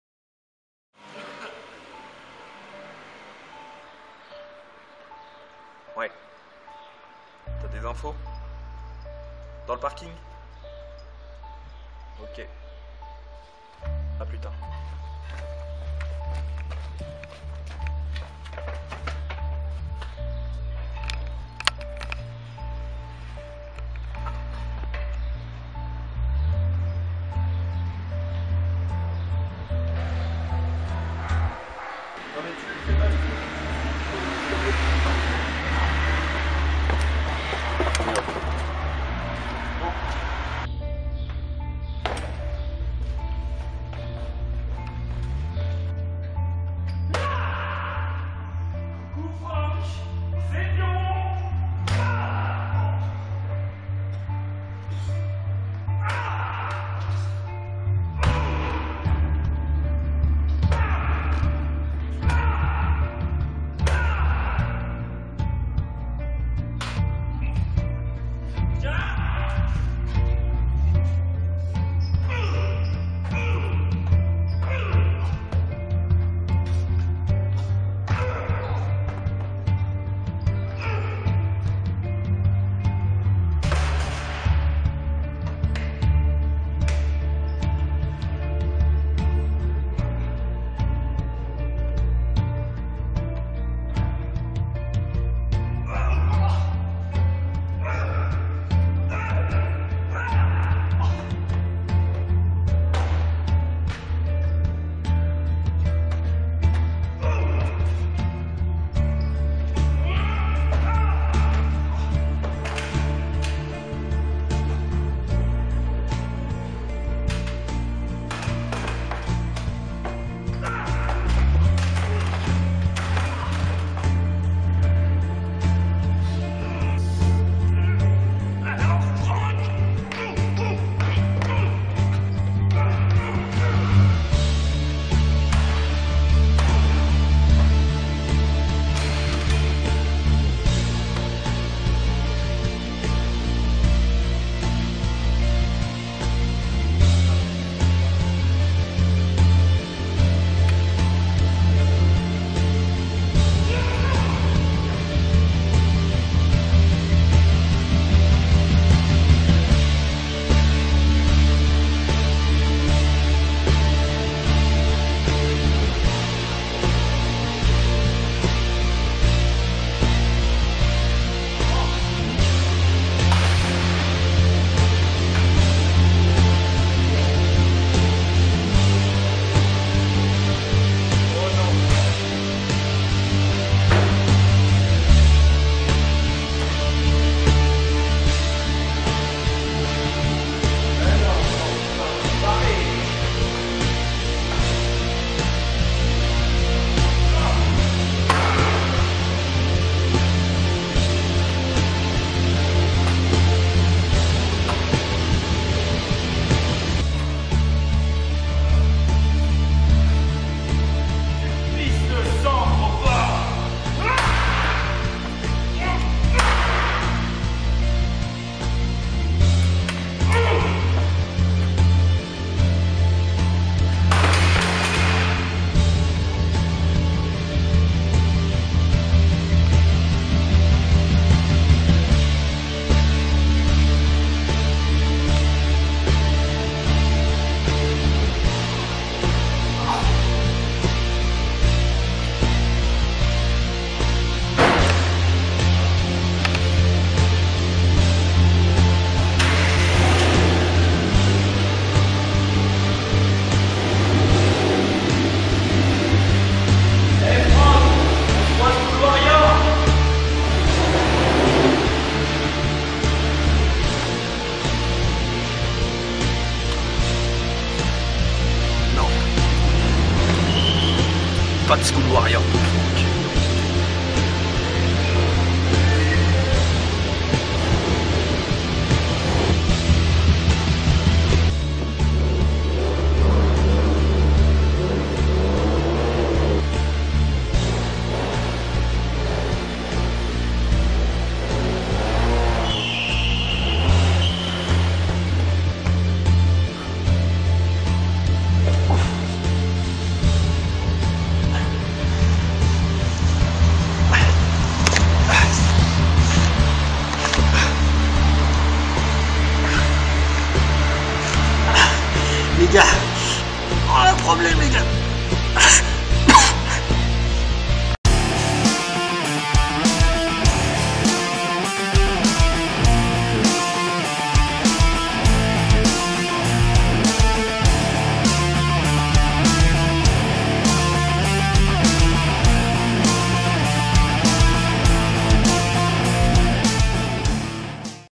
ON A UN PROBLEME LES GARS - Promo School Warriors - catch france - catch newz
Le Catcheur IRON PAUL, du club de catch de E.C.A. - LYON, attaque violemment Franck aka FENRIR, de La salle de catch de E.C.A. Rumilly (74150). En prévision du Show d'école de ce Vendredi 13 "SCHOOL WARRIORS", qui aura lieu à Rumilly (74150), Franck aka FENRIR, n'aura pas besoin d'être bercé pour bien dormir.

Si tu as été jusqu'ici écris CRAKIGNOU, dans l'espace commentaires.

Liens utiles :

La page Facebook de ECA - Ecole de Catch Aya

La page Twitter de ECA - Ecole de Catch Aya